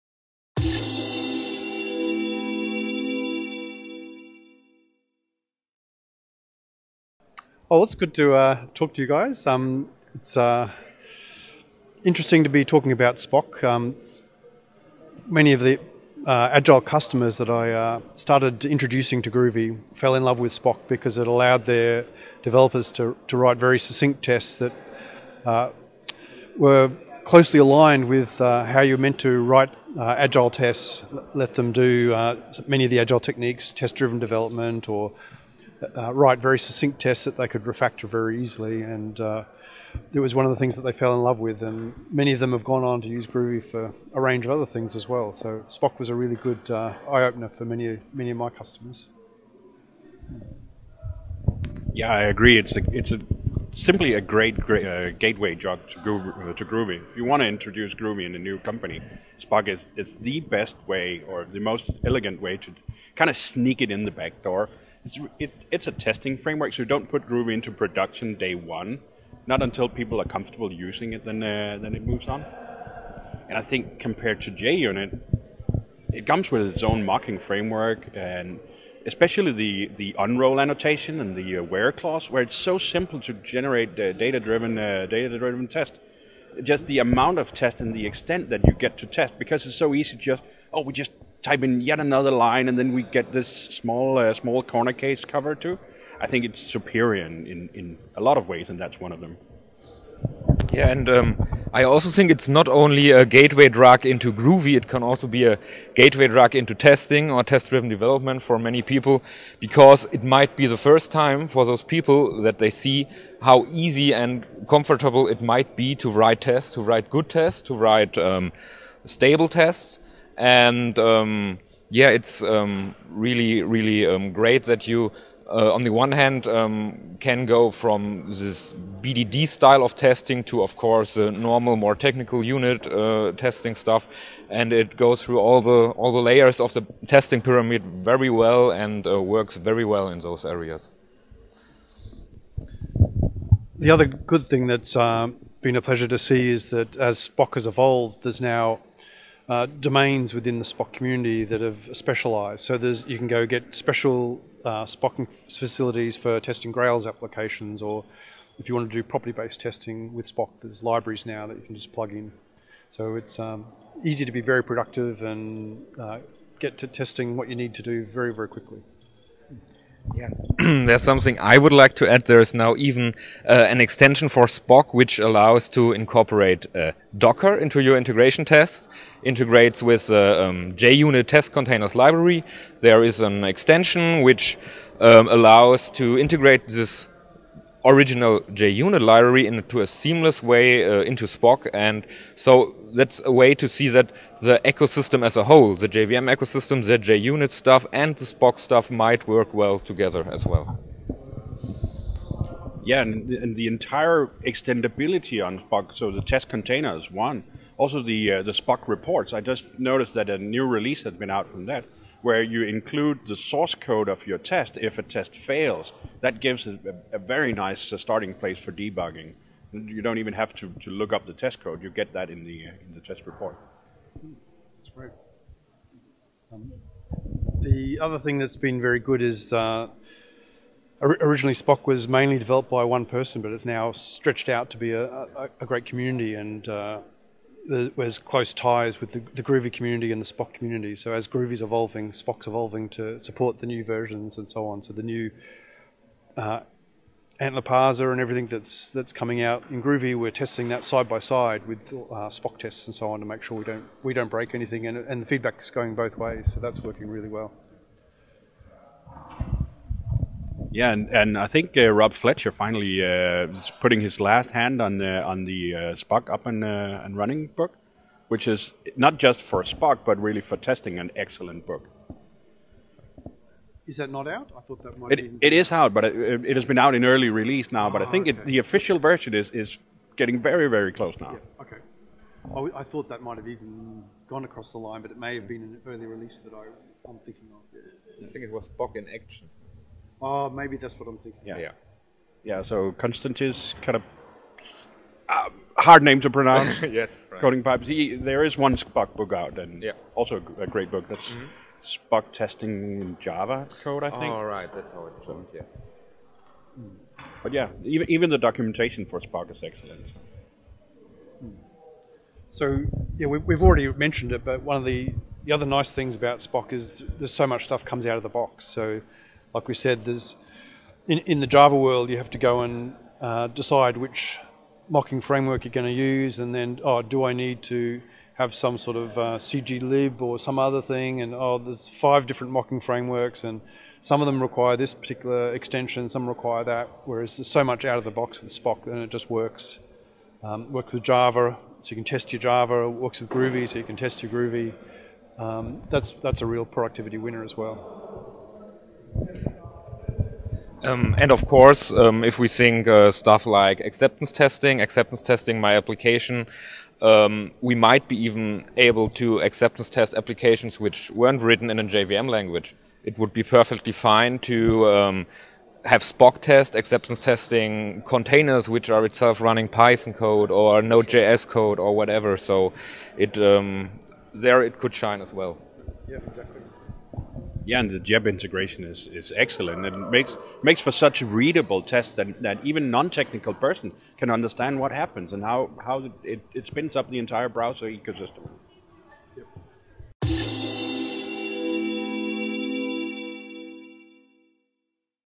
Jacob Aae Mikkelsen, Kevin Wittek and Paul King - GR8Conf 2017 (interview by Autentia)
Jacob is a senior software architect at Cardlay A/S He has been working full time with Groovy based technologies the last many years, including positions at Gennemtænkt IT and the LEGO Group.
As a hobby, he regularly teach a course at the University of Southern Denmark, including the hands on: "Web development with Groovy and Grails". He's been on the crew arranging GR8conf Europe for several years, and regularly, blogging the Grails Diary with news from the community.

Kevin is a Java developer and JVM fanboy since being introduced to the language back in school 12 years ago. Found his own company during his university years, developing mobile apps and client-server applications, which lead to his discovery of Grails and the Groovy ecosystem. Fell in love with TDD because of Spock. Nowadays Kevin is working at the cyber security company GDATA and focuses on topics such as software craftsmanship, infrastructure as code and continuous integration pipelines, trying to make the world a better place one container at a time.

Paul has been contributing to open source projects for nearly 30 years and is an active committer on numerous projects including Groovy, GPars and Gradle. Paul speaks at international conferences, publishes in software magazines and journals, and is a co-author of Manning’s best-seller: Groovy in Action, 2nd Edition.